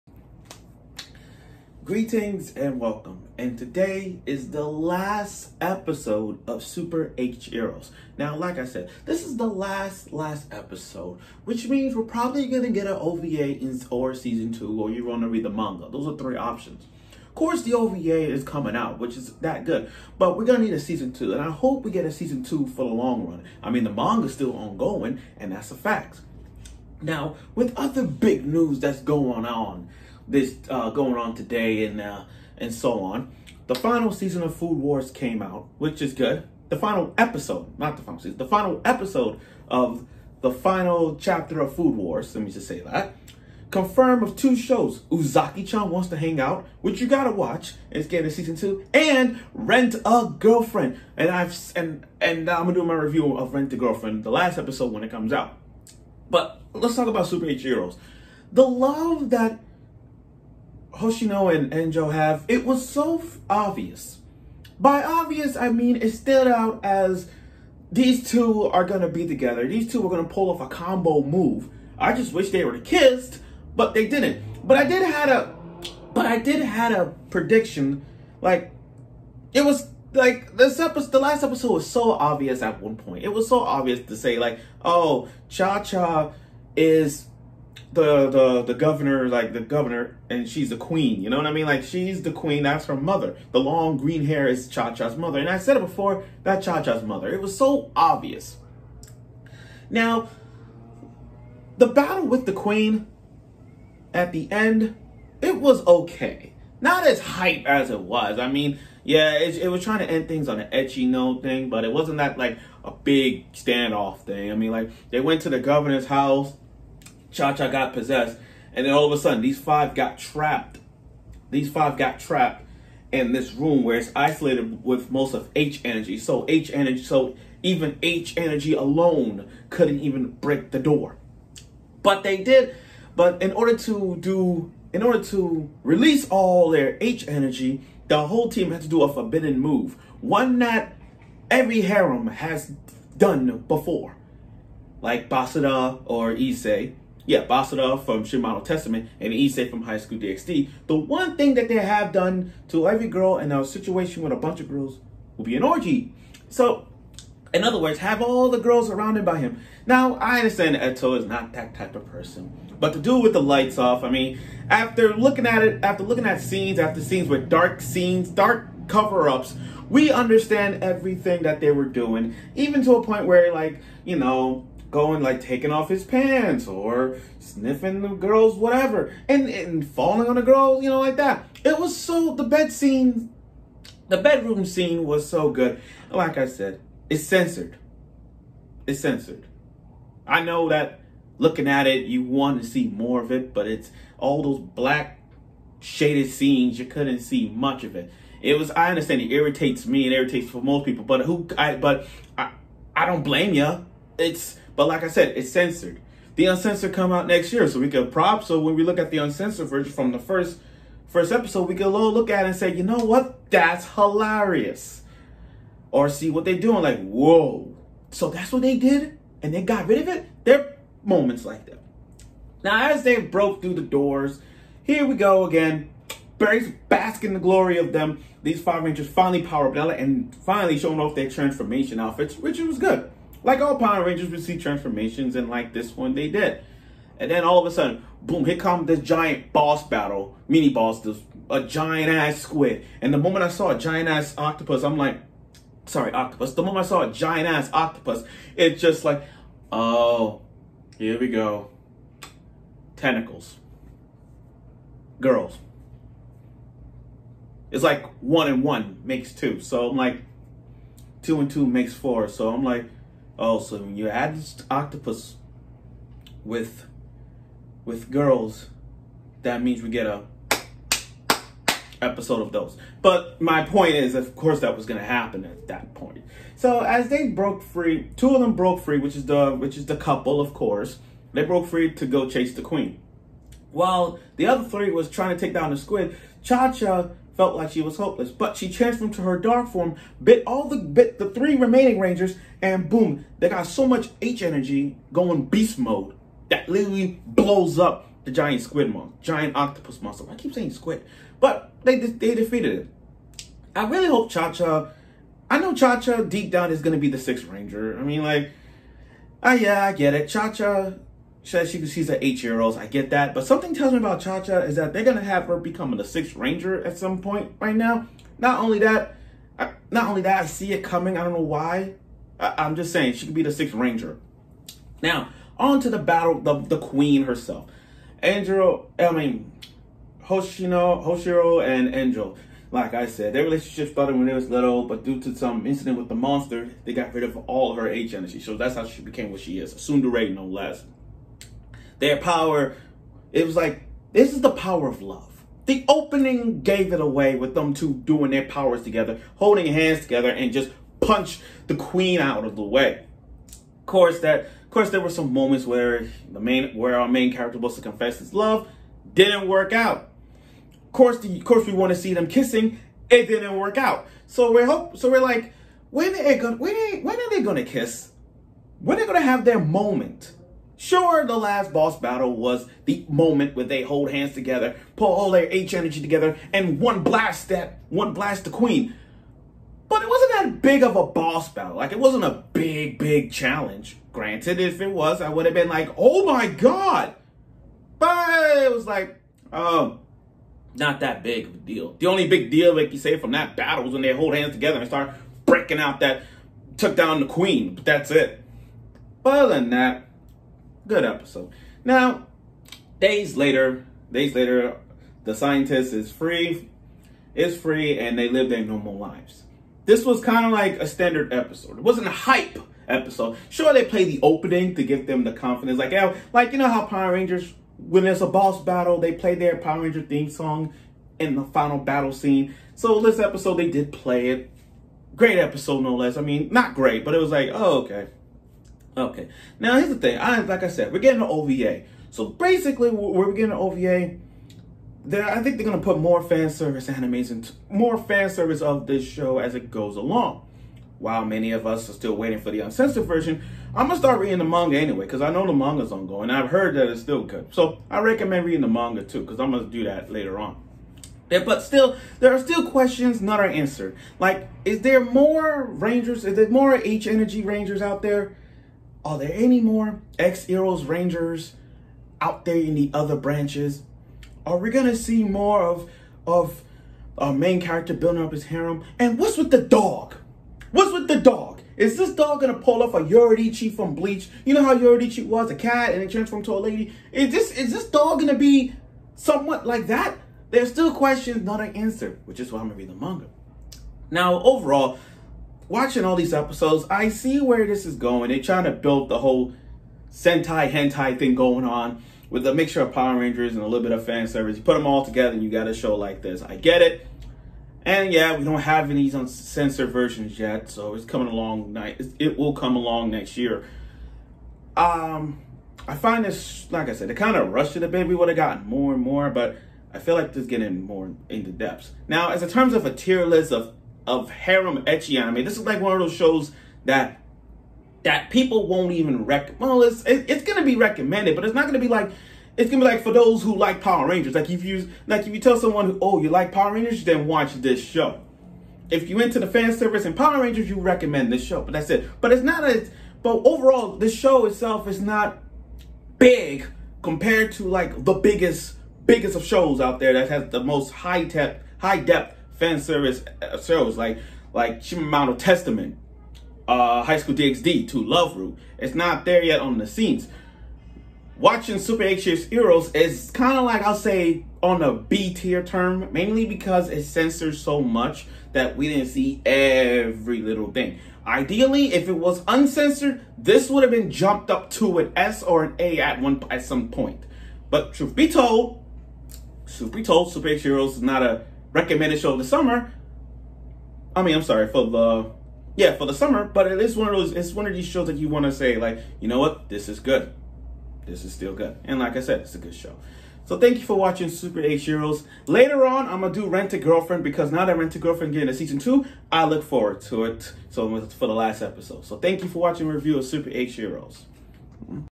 Anime Review of Super HxEros episode 12 (The last episode)
The ending was decent. It wasn’t that bad. I hope we get a season 2 coming along.
Overall- B tier show
Rating 7.5/8 out of 10